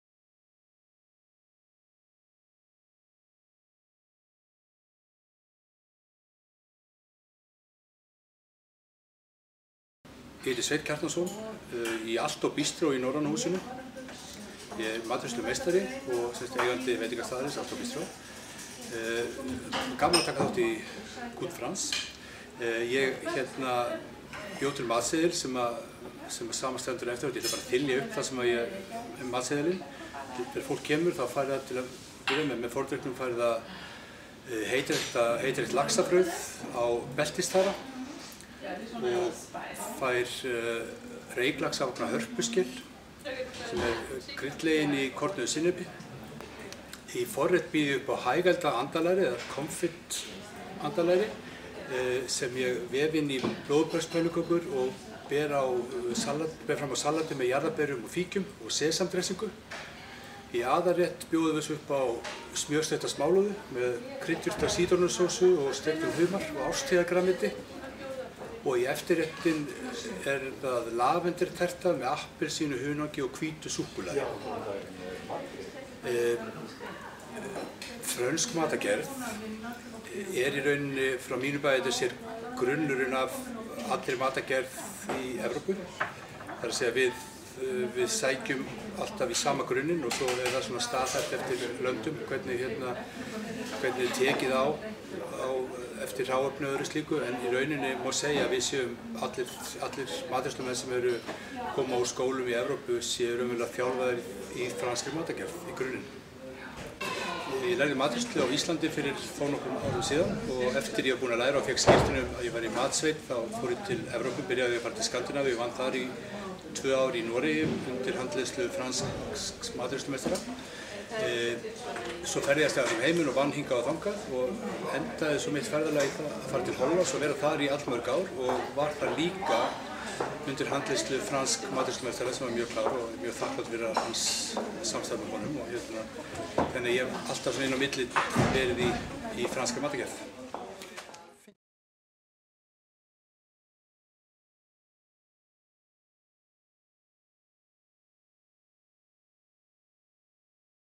AALTO Bistro
Sveinn Kjartansson, chef at AALTO Bistro in the Nordic House  in Reykjavík, participant in the Goût de France / Good France celebration the 21st March 2016, tells about his background, his menu and the French influence in his cuisine.